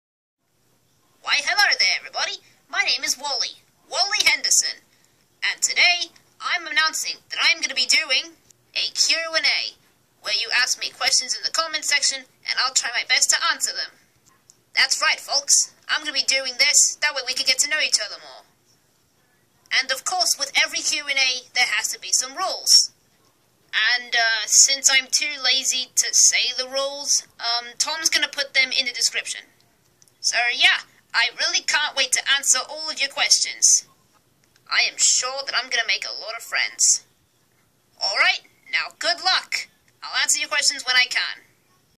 Wally Henderson's Q&A Announcement (Q&A IS CLOSED)
Rules:

1.) You can ask Wally 1, 2, or 3 questions as long as they're different questions and not all the same.

2.) Don't ask Wally any sexual, political, or religious questions.

3.) You can also give him riddles if you know any, just DON'T give him the answer to your riddle. Let him figure it out.

4.) Be specific with your questions.

Have fun!!